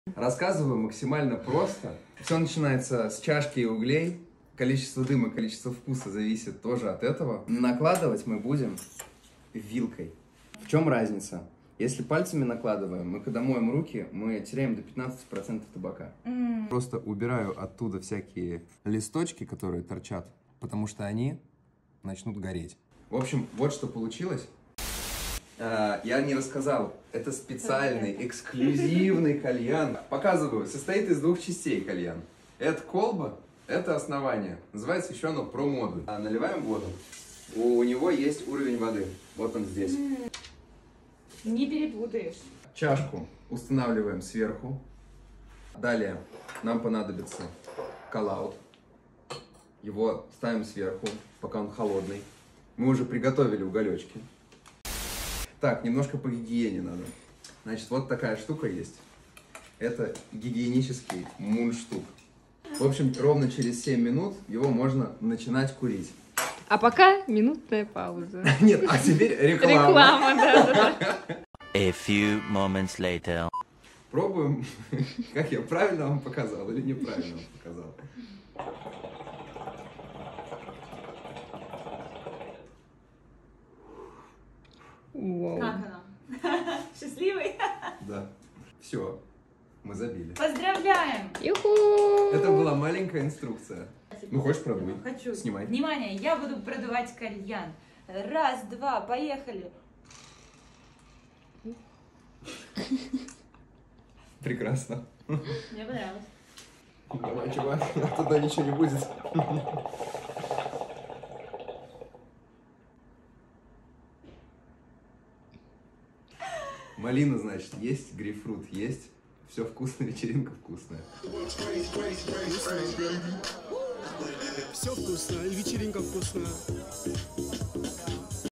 Как забить кальян для новичка? Инструкция NANOSMOKE 2022
Видео для тех, кто купил кальян, но не знает как его использовать.
Или собирается. По остальным вопросам ватсап